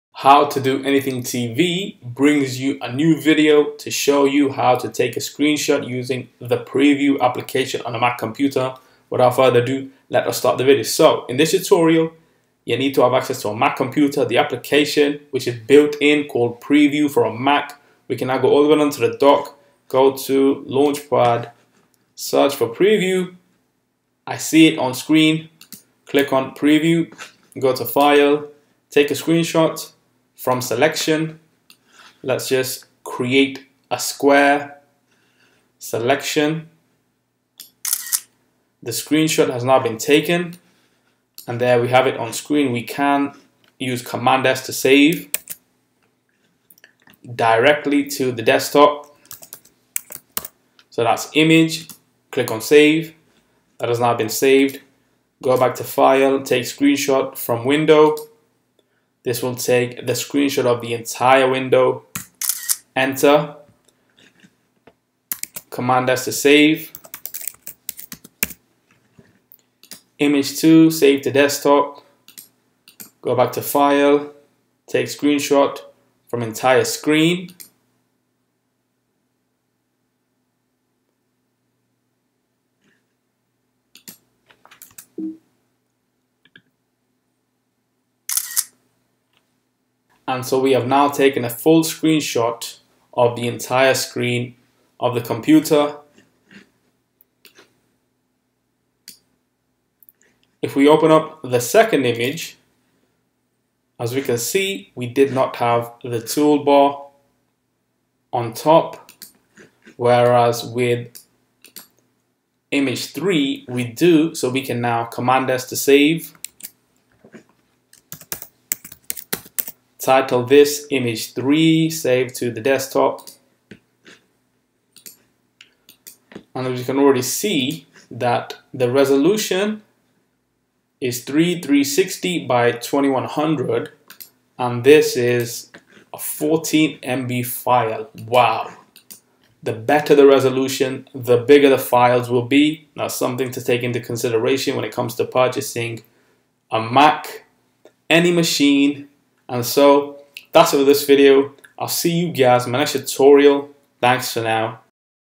How to CAPTURE a Screenshot Using the Preview Application On a Mac - Basic Tutorial | New
How to Take a Screenshot Using the Preview Application On a Mac - Basic Tutorial | New @ComputerScienceVideos